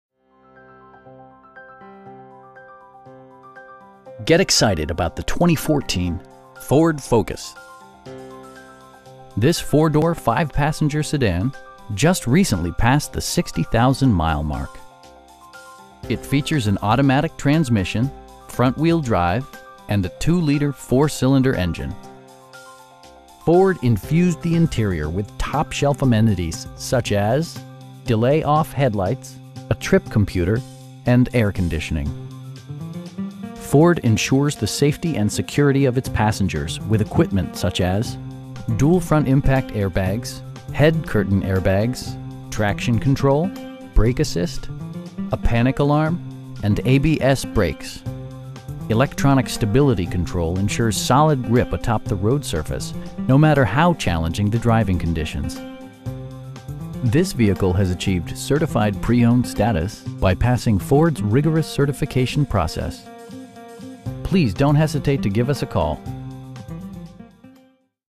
2014 Ford Focus SE in Lancaster, CA 93534
Get excited about the 2014 Ford Focus. This 4 door, 5 passenger sedan just recently passed the 60,000 mile mark! It features an automatic transmission, front-wheel drive, and a 2 liter 4 cylinder engine. Ford infused the interior with top shelf amenities, such as: delay-off headlights, a trip computer, front bucket seats, telescoping steering wheel, power door mirrors, cruise control, and air conditioning. Ford ensures the safety and security of its passengers with equipment such as: dual front impact airbags, head curtain airbags, traction control, brake assist, a panic alarm, and ABS brakes. Electronic stability control ensures solid grip atop the road surface, no matter how challenging the driving conditions. This vehicle has achieved Certified Pre-Owned status, by passing Ford's rigorous certification process. We pride ourselves on providing excellent customer service. Please don't hesitate to give us a call.